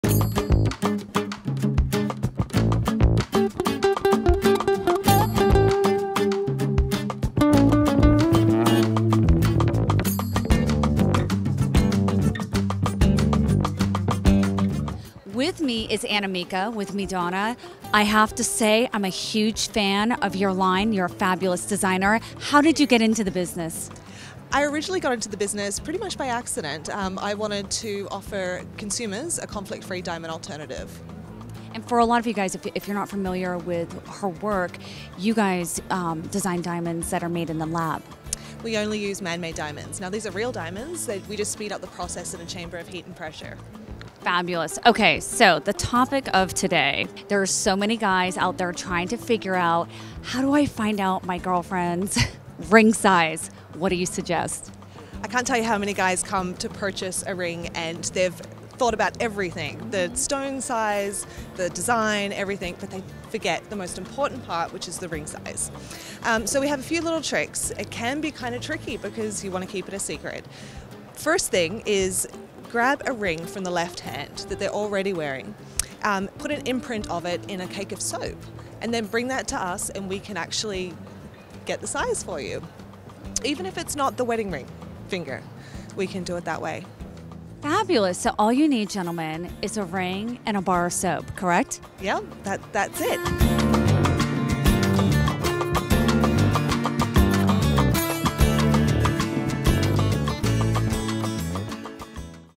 How to Find out Her Engagement Ring Size
The Engagement 101 Minute - Episode 18 - In this week episode, Anna-Mieke from Miadonna gives you a neat trick to get her ring size!